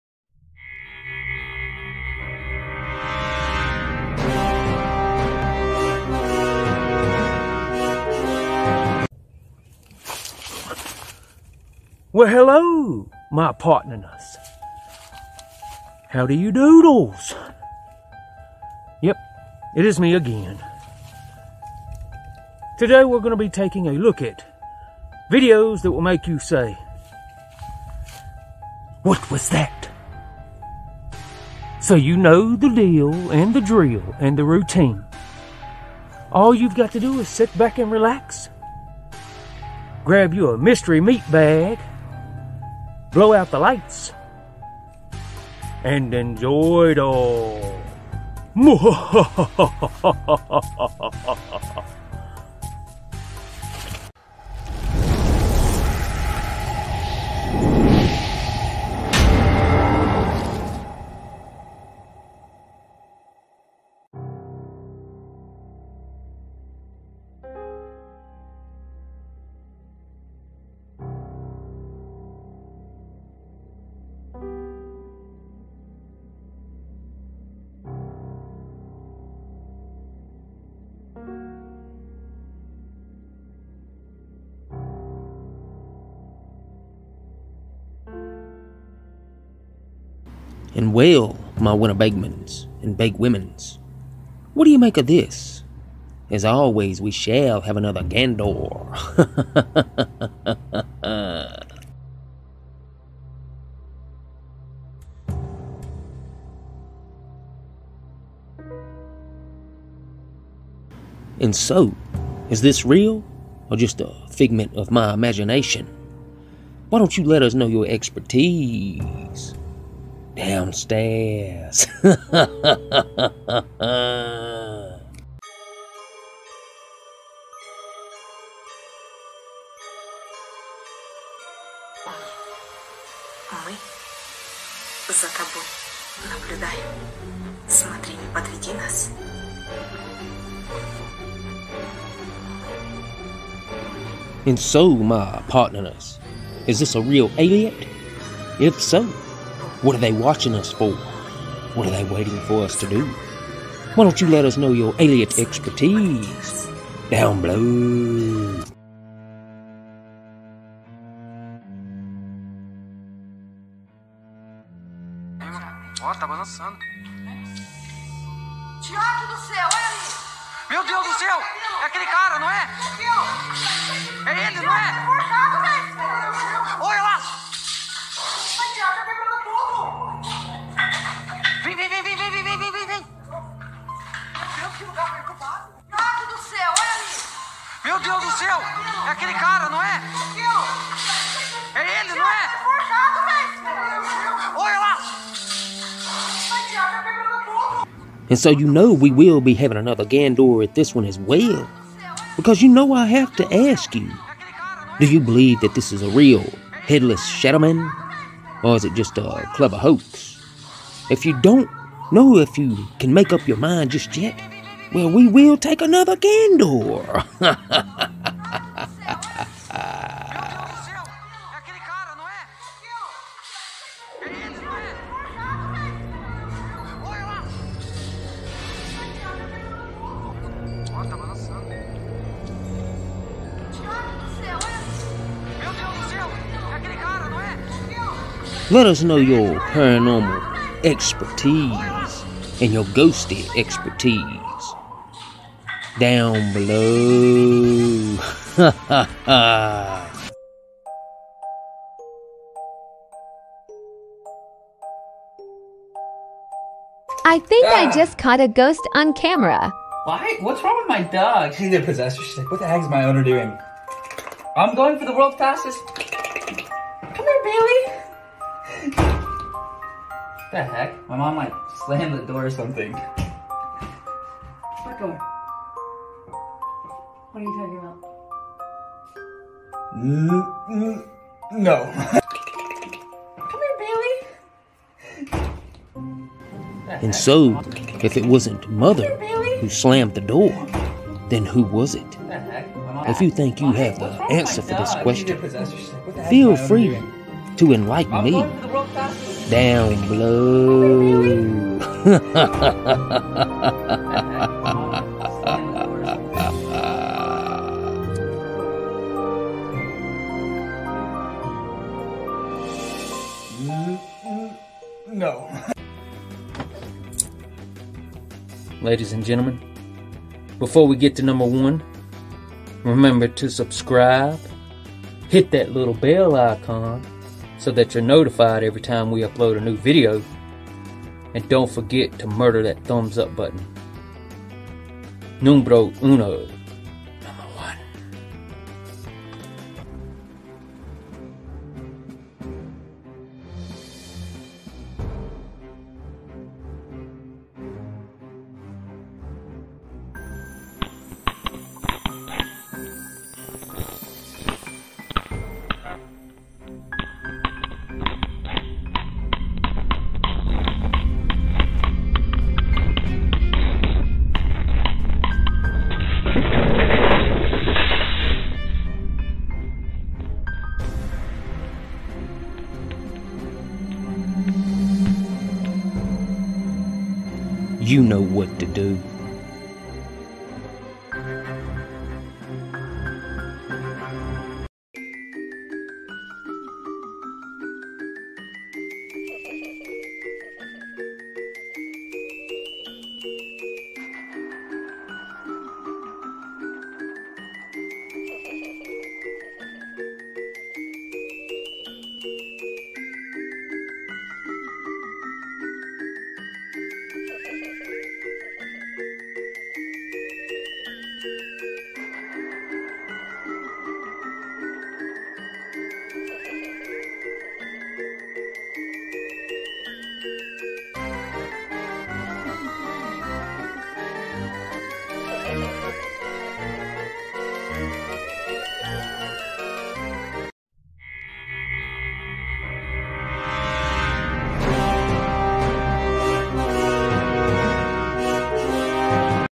Videos That Make You Say What Was That?
#5. Ovni
paranormal_horror_1/Instagram

#4. We Are Watching You
shareza210/Instagram

#3. Headless ShadowMan
BIG DATA AMBURADUL/youtube

#2. I Think My Mom Did It
@mikepanero/Tik Tok

#1. The Trap
@iloveghosties/Tik Tok

$kingX1979

Thanks Again

   Sincerely Yours, King X